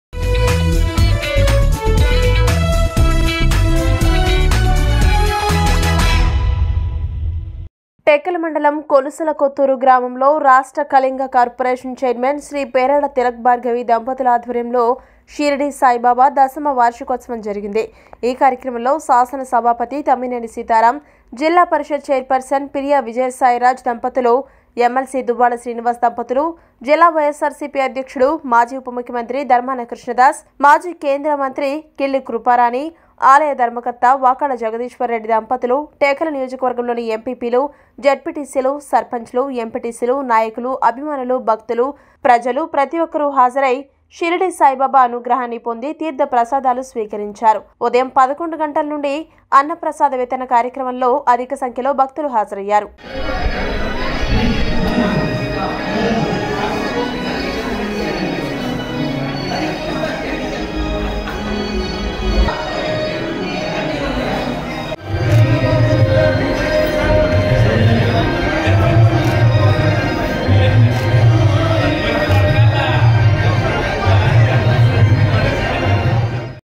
కళింగ కార్పొరేషన్ చైర్మన్ పేరాడ తిలక్-భార్గవి ఆధ్వర్యంలో షిరిడి సాయి దశమ వార్షికోత్సవం..
@truthnewstelugu కళింగ కార్పొరేషన్ చైర్మన్ పేరాడ తిలక్-భార్గవి ఆధ్వర్యంలో షిరిడి సాయి దశమ వార్షికోత్సవం..